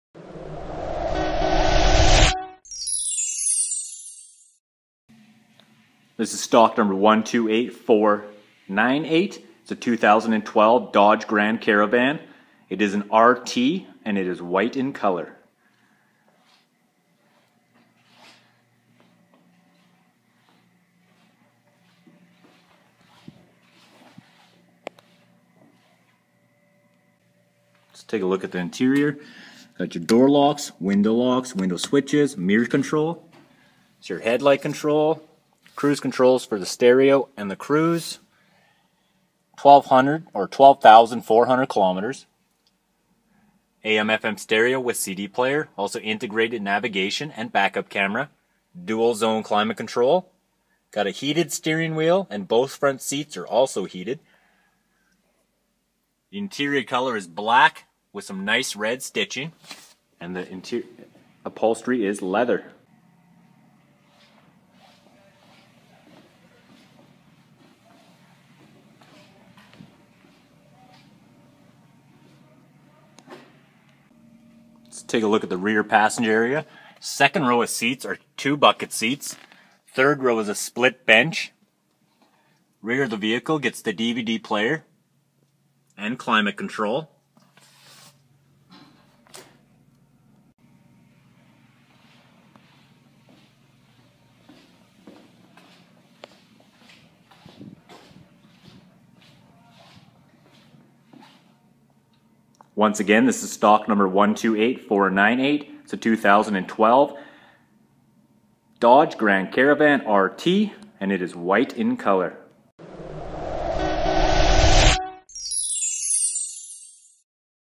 Used 2012 Dodge Grand Caravan R/T Mini-Van - Loaded with options Stock #128498
Used 2012 Dodge Grand Caravan R/T Mini-Van - Loaded with options - White exterior, Black leather interior. Ask about our delivery service to Calgary, Edmonton, Medicine hat, Fort McMurray, Grande Prairie, Red Deer

This is a 2012 Dodge Grand Caravan 4dr Wgn R/T 4 Door Mini-Van Passenger Front Wheel Drive with 6-Speed A/T transmission White[Stone White] color and Black Interior interior color.